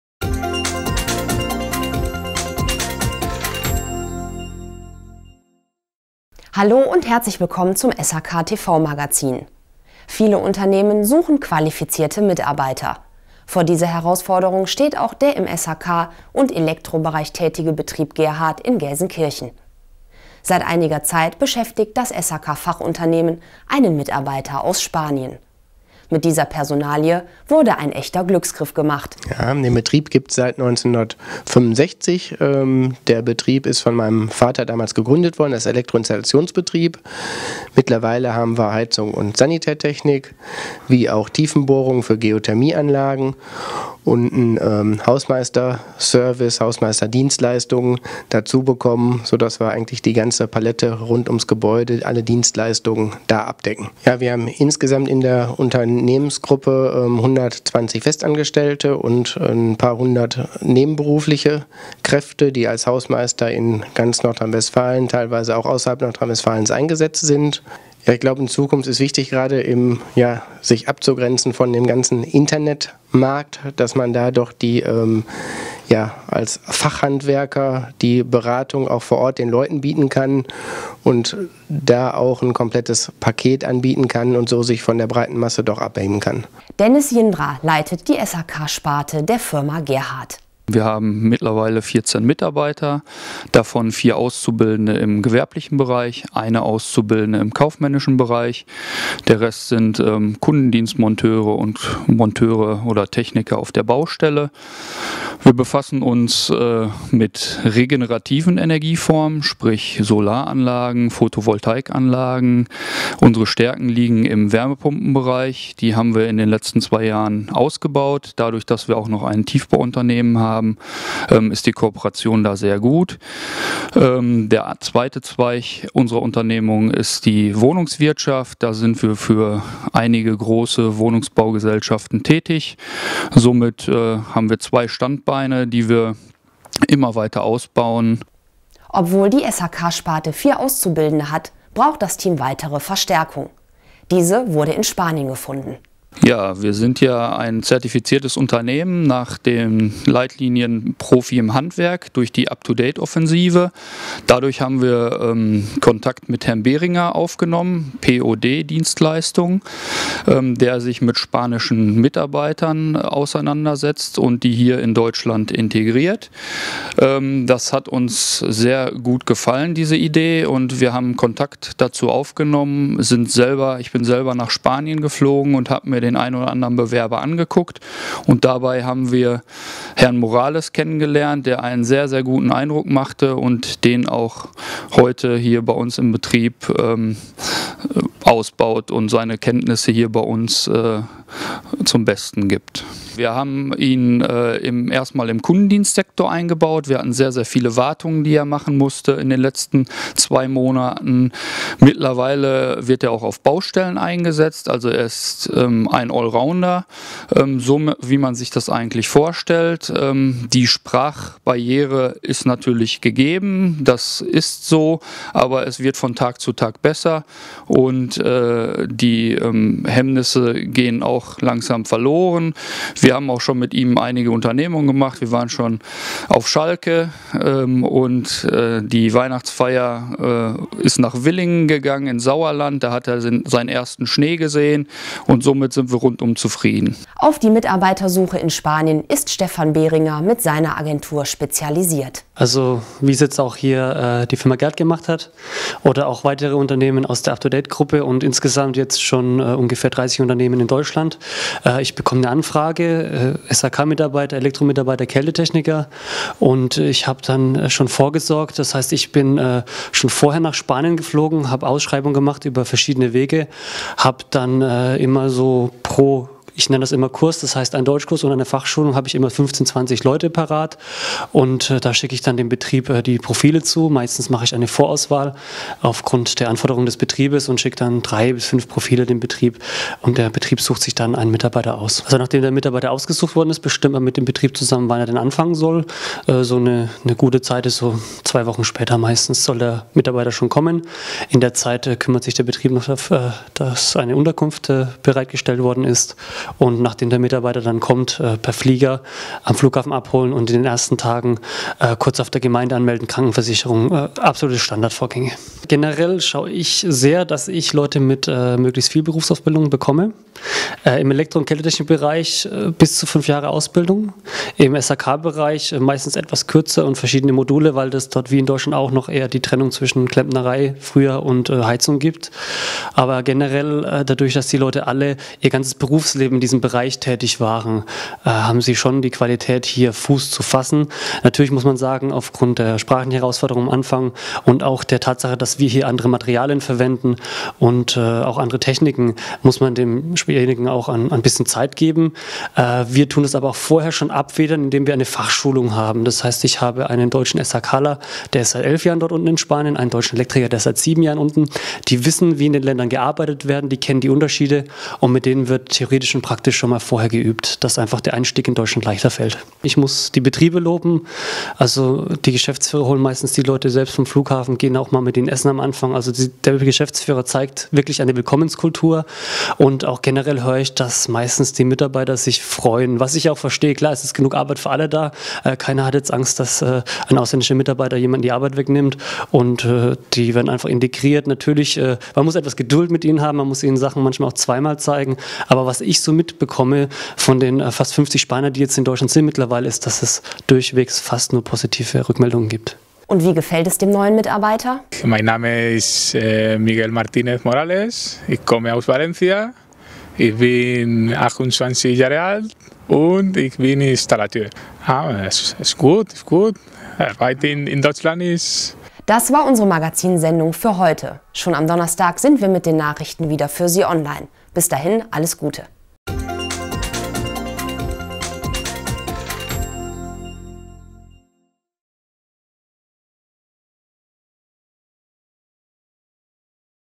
SHK-TVmagazin: Betrieb Gerhardt setzt auf qualifizierte Mitarbeiter aus dem Ausland
Wie bekomme ich qualifizierte Mitarbeiter? Vor dieser Herausforderung steht auch der Gelsenkirchener SHK- und Elektrobetrieb Gerhardt. Betriebsinhaber Christian Gerhardt ist in dieser Thematik neue Wege gegangen und hat einen Mitarbeiter aus Spanien eingestellt. Seine bisherigen Erfahrungen sind durchweg positiv.